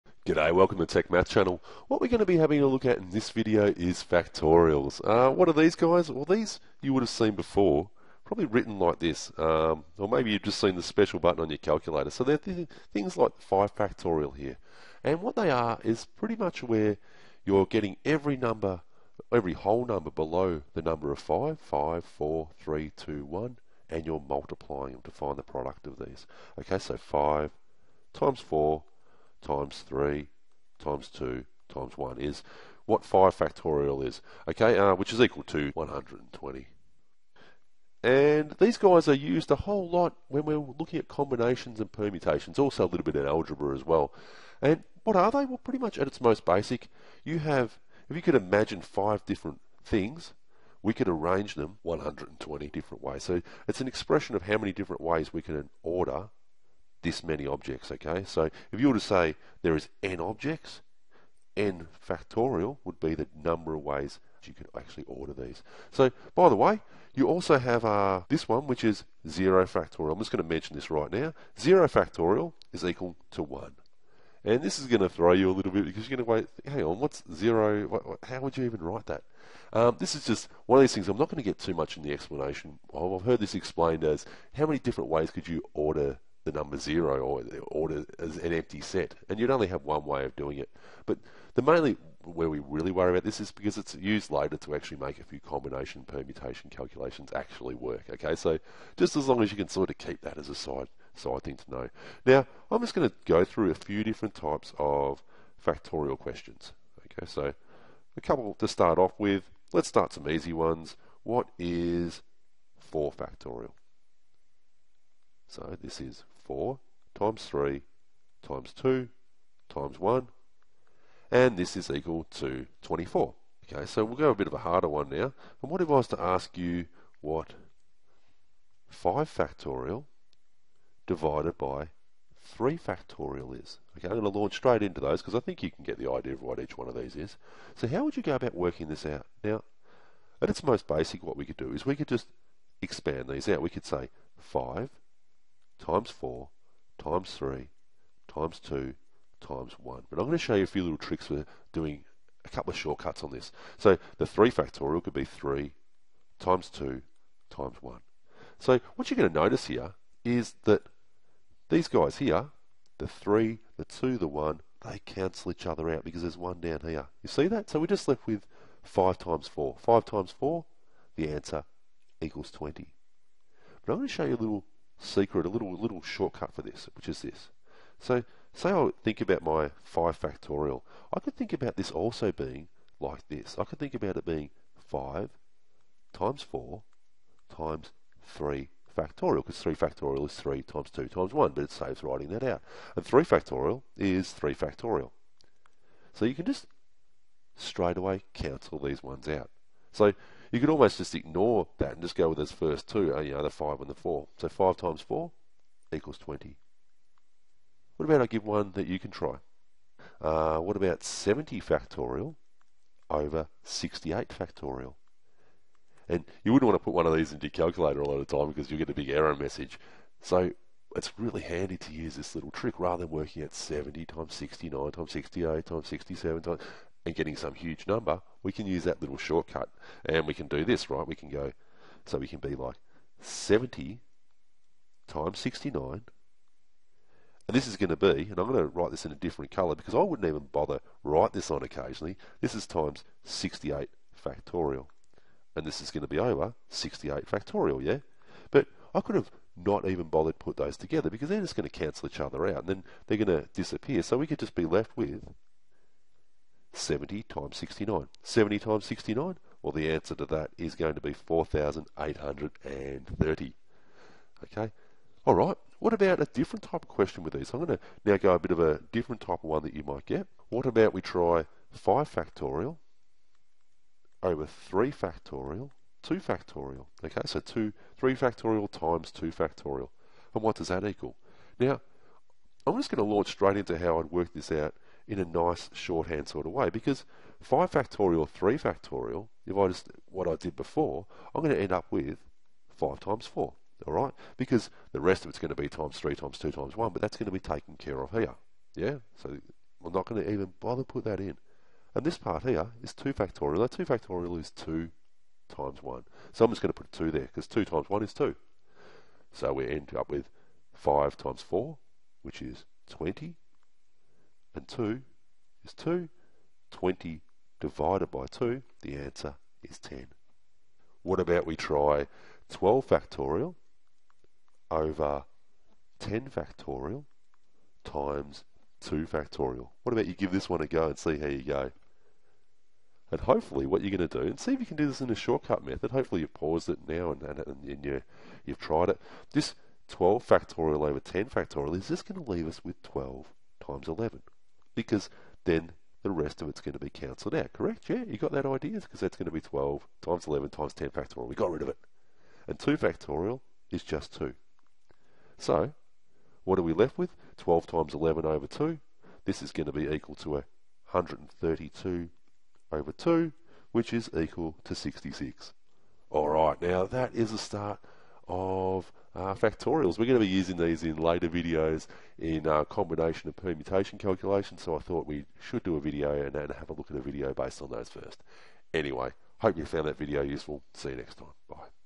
Factorials shortcuts
Factorials are easy!  This basic video lesson with show you the basics of factorials as well as some shortcuts in calculations involving factorials.